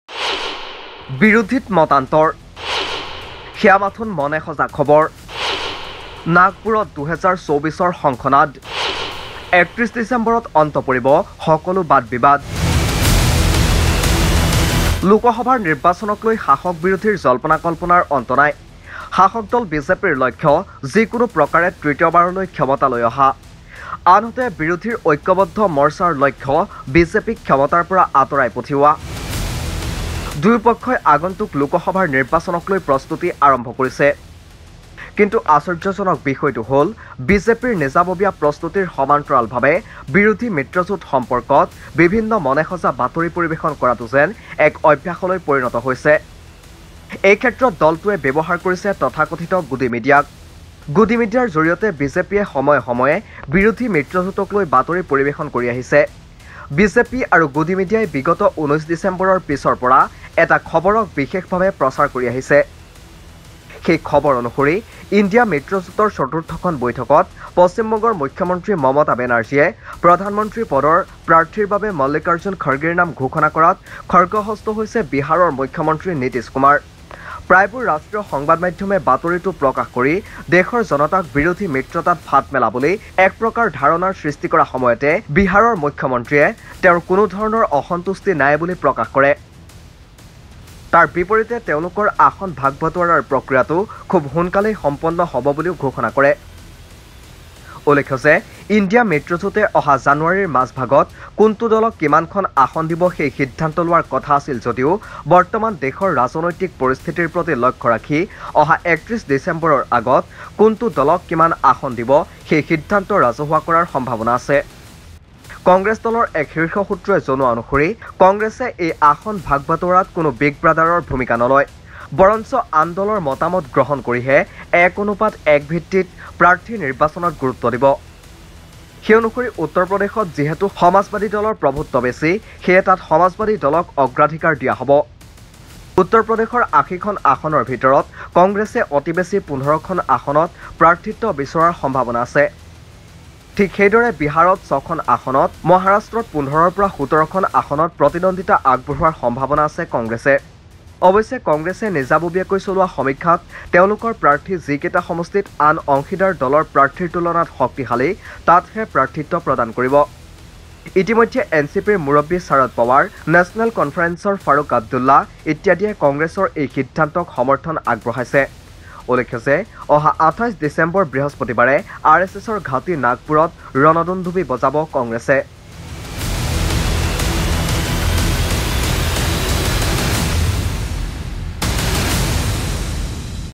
৩১ ডিচেম্বৰত অন্ত পৰিব সকলো বাদ-বিবাদ !
বিৰোধীত মতান্তৰ ! সেয়া মাথোন মনে সজা খবৰ ! নাগপুৰত ২০২৪ ৰ শংখনাদ। ৩১ ডিচেম্বৰত অন্ত পৰিব সকলো বাদ-বিবাদ !